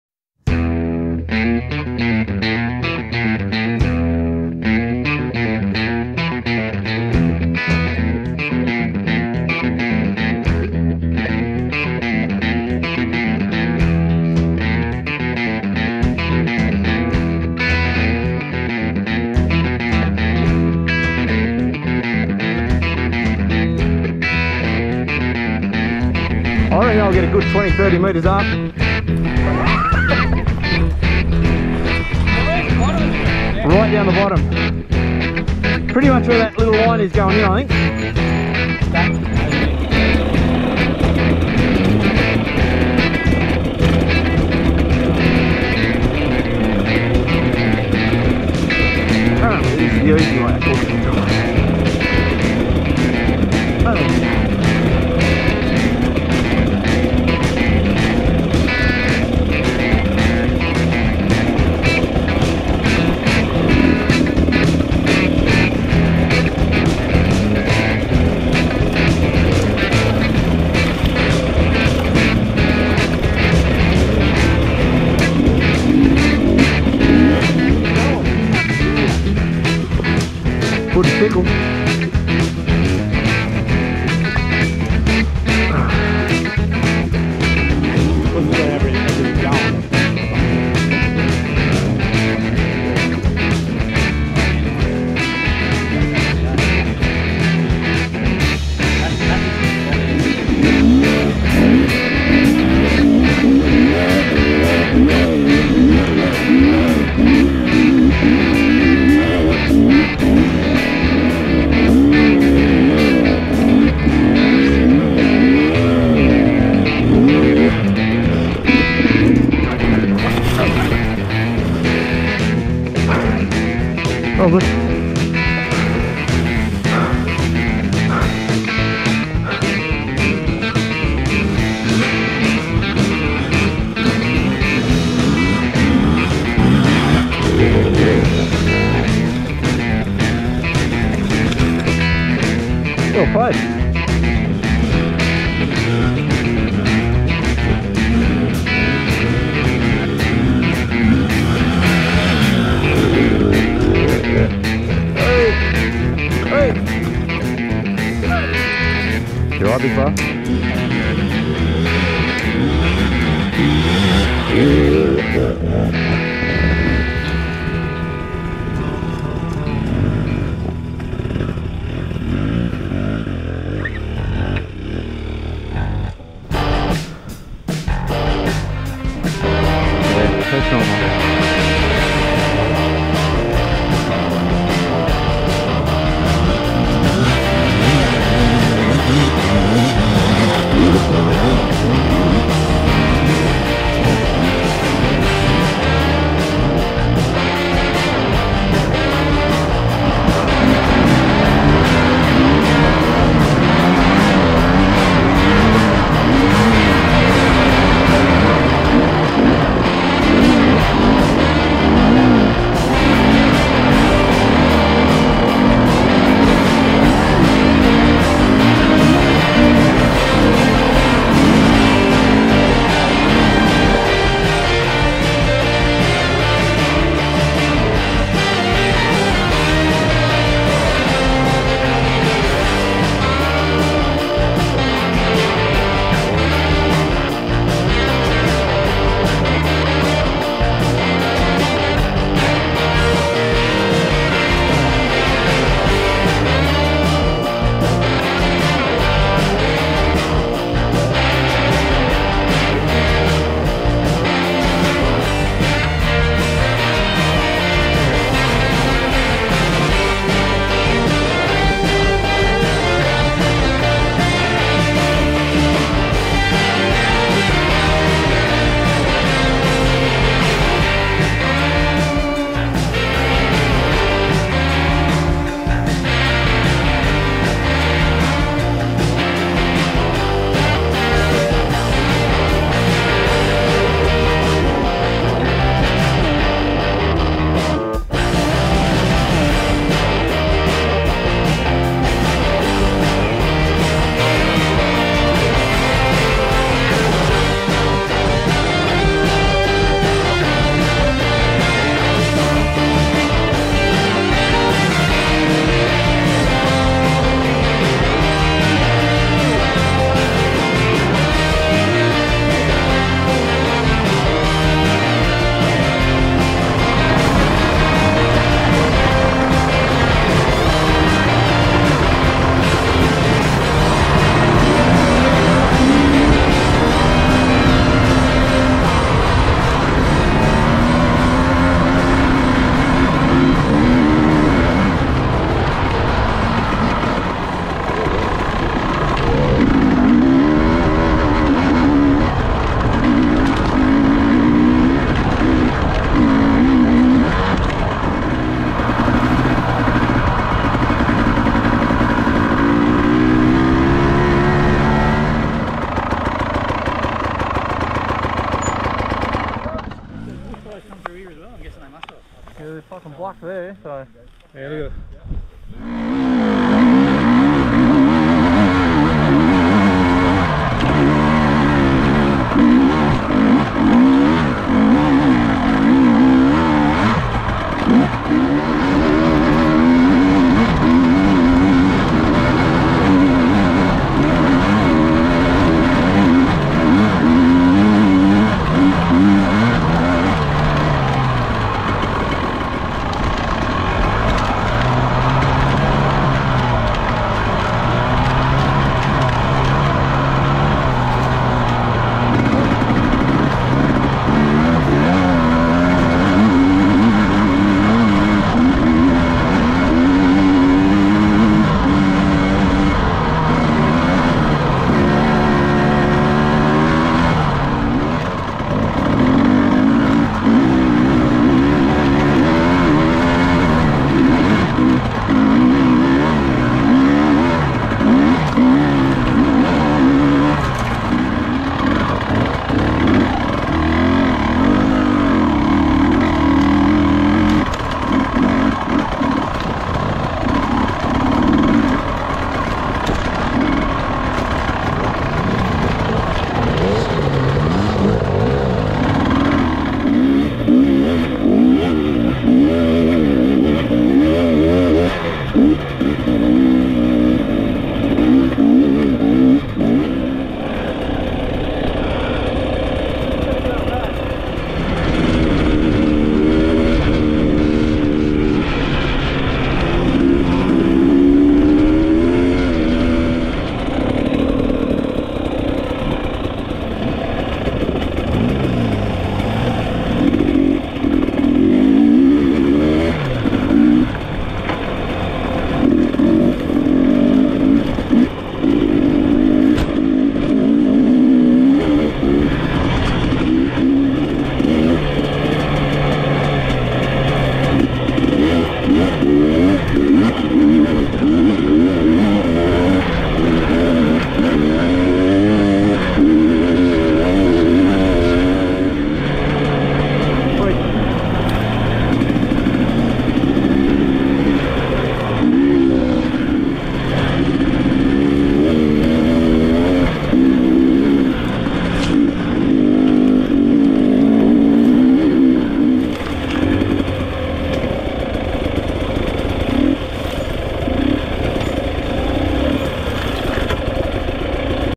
Pork chop hill
Went south in search of the famous Pork Chop Hill and it didnt disappoint. What a climb the thing is a bloody monster, the surface is shale rock and getting traction is almost impossible but what a blast trying to get up. Sorry for the drone videos it was windy as hell and powerlines straight above the trail so limited opportunity to get the footage i was looking for.
8 riders, 6 blokes tried 5 made it to the top.